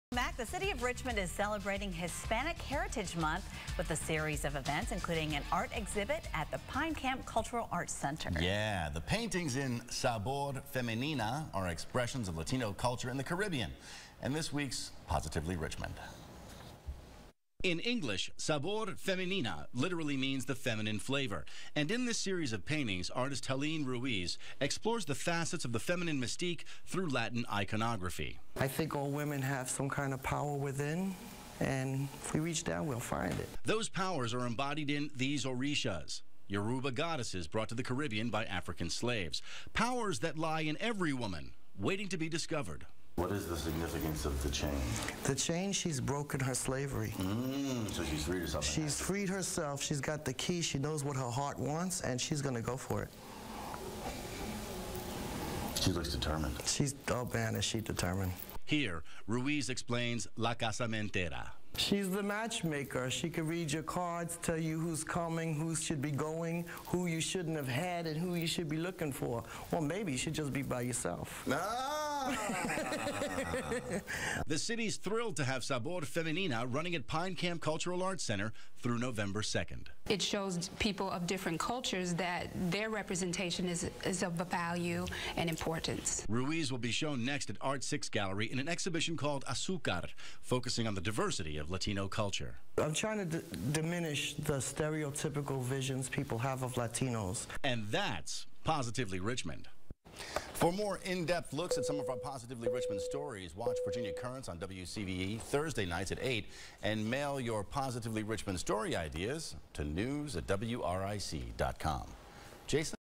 Shaunn Casselle in "Sabor Feminina" interview
Art exhibit for Hispanic Heritage Month-- Shaunn Casselle, curator